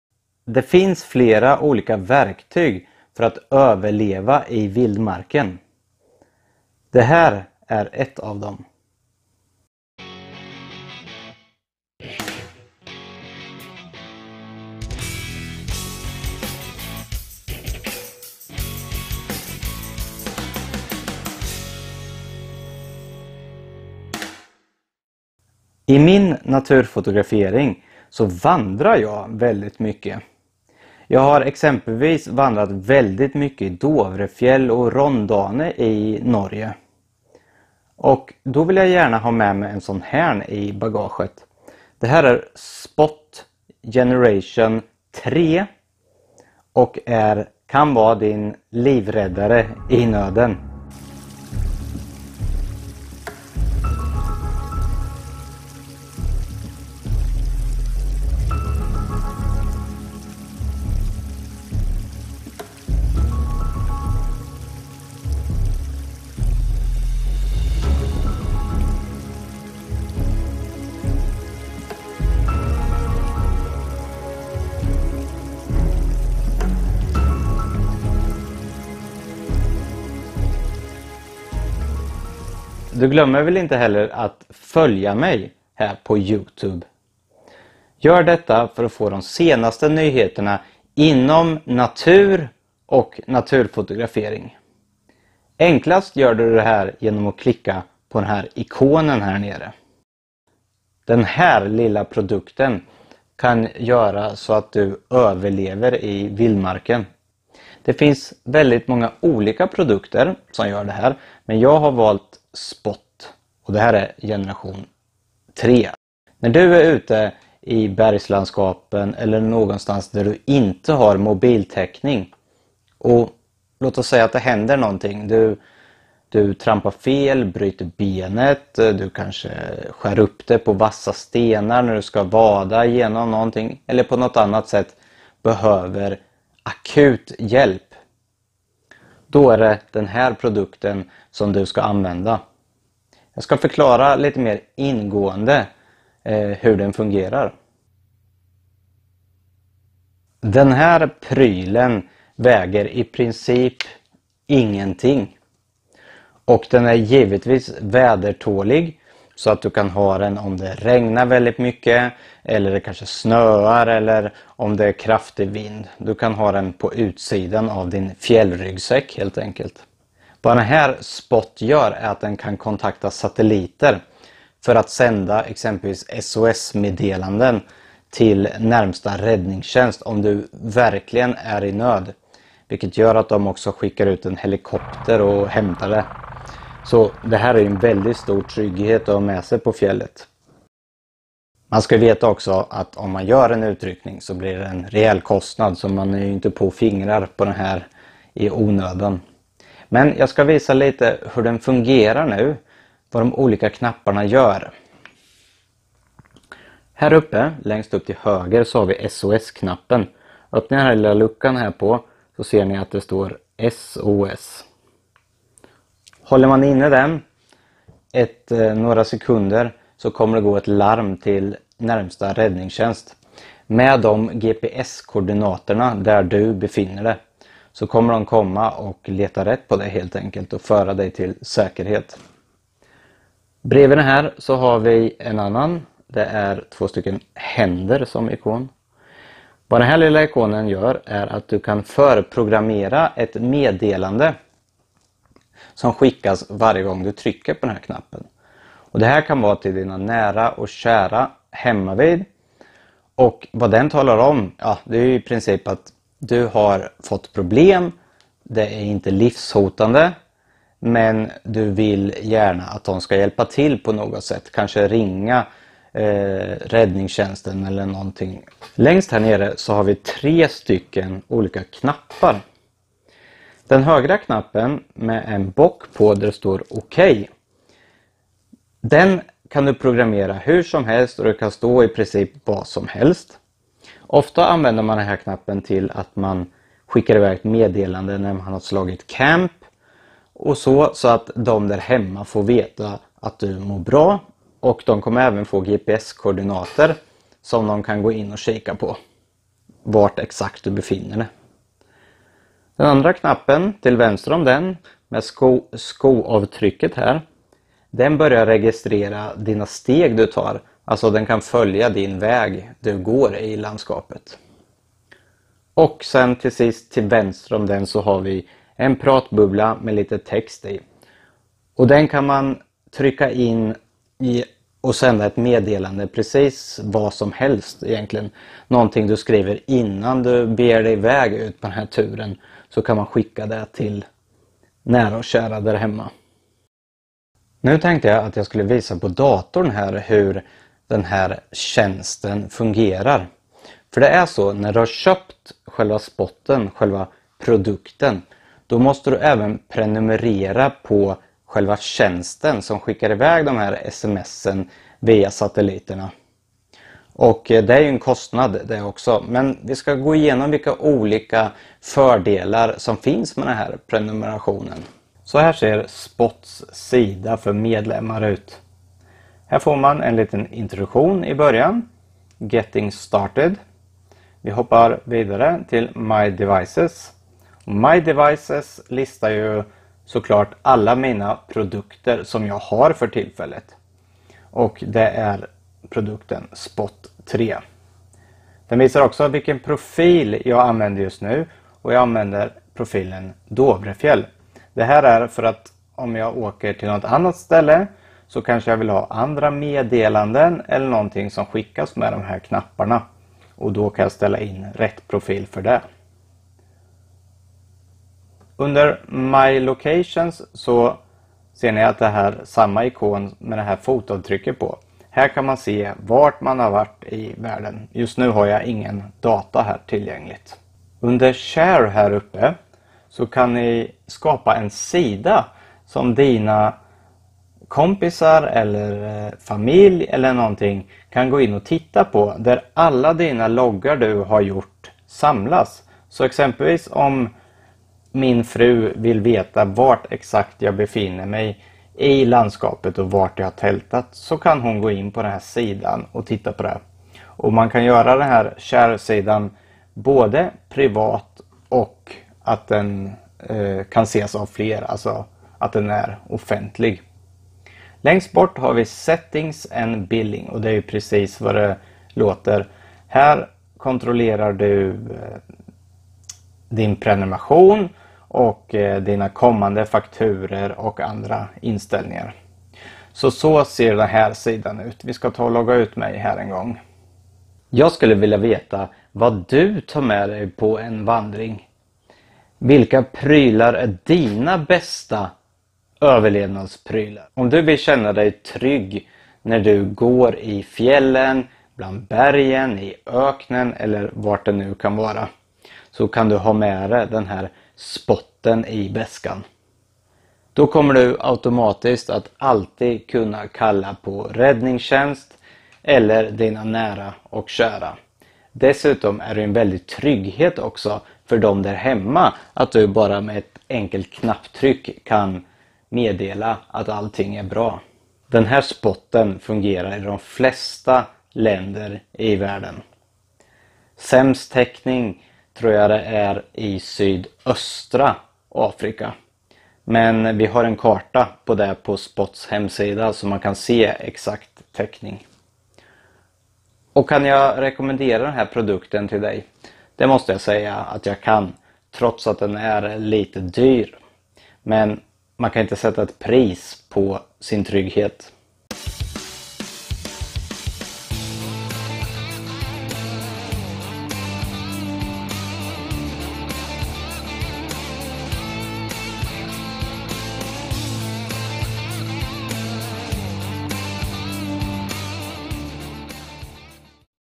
Överlevnad i vildmarken - Spot Gen 3
Överlevnad i vildmarken heter det senaste avsnittet. Här kommer jag att illustrera Spot. Generation 3! En GPS och textmaskin sin kommer att hålla dig trygg i nordens vildmark. Med hjälp av en GPS och kontakt med satelliter kan Spot användas även när det inte finns telefontäckning och på så sätt rädda ditt liv.

Men du kanske bara vill berätta för dina nära och kära att du har det bra och arr du överlever i skogen eller på fjället, det går bra det också. Ofta kan vi läsa och höra om olika överlevnadstips, det är bra! Spot är ytterligare ett starkt överlevnadstips! Med hjälp av satelliterna kan du överleva i vildmarken!